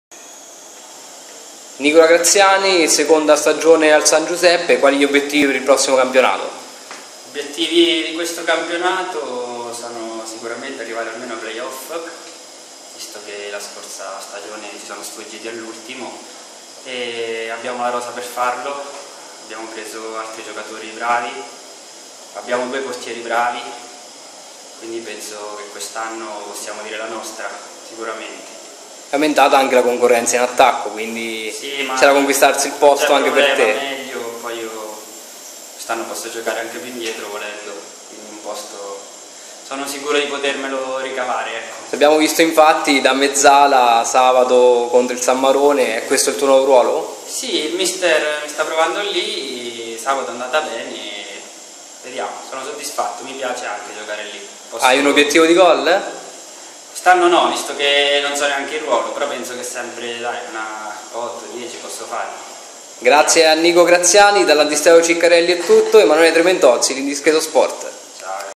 Terza Categoria Civitanova. Intervista con Nico Graziani, attaccante del San Giuseppe
Intervista post allenamento del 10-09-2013